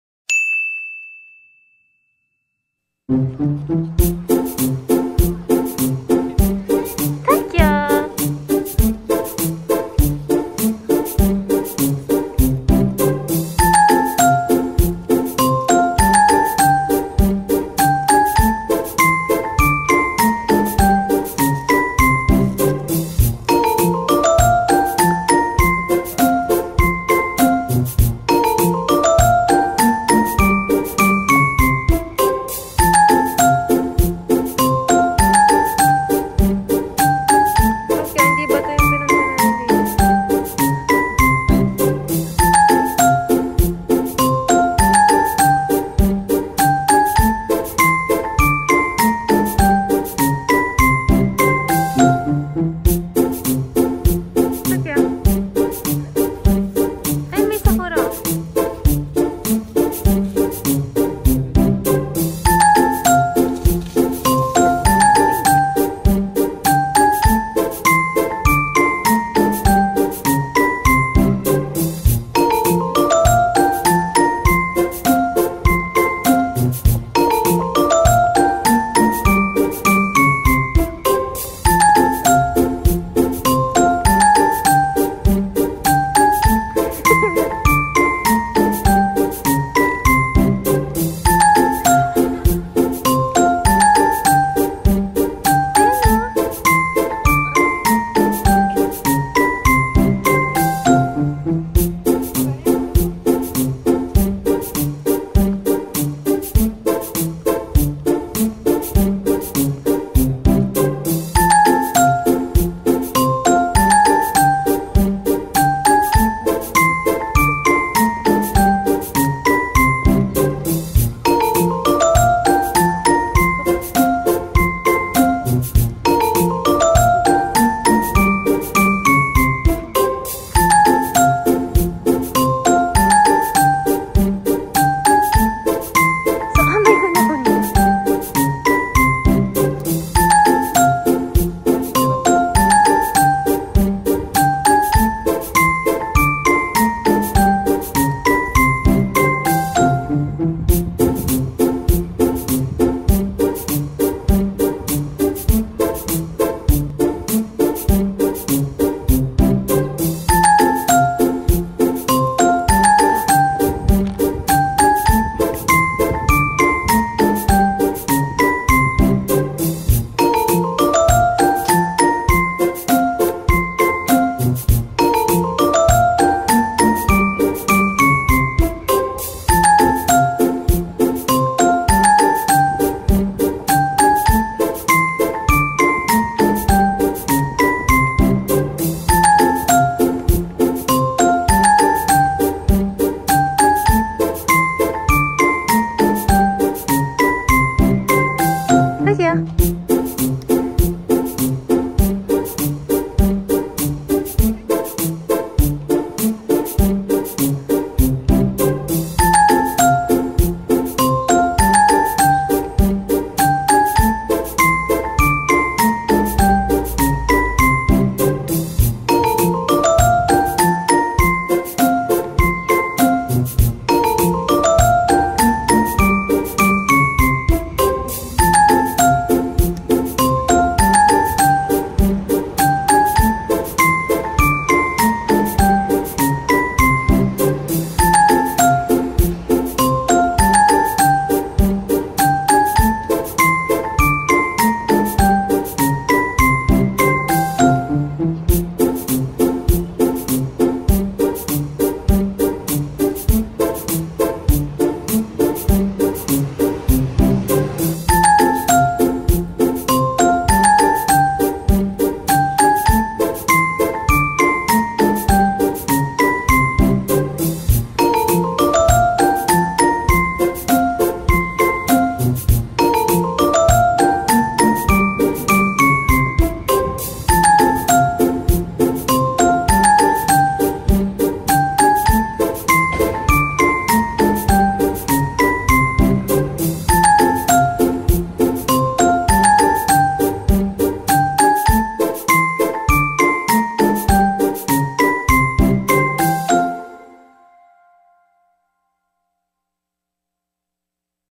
[SHIBA INU VLOG] CHERRY BLOSSOMS IN ODAIBA
HELLO!

MY NAME IS TOKIO
IM A FEMALE SHIBA INU
FROM TOKYO, JAPAN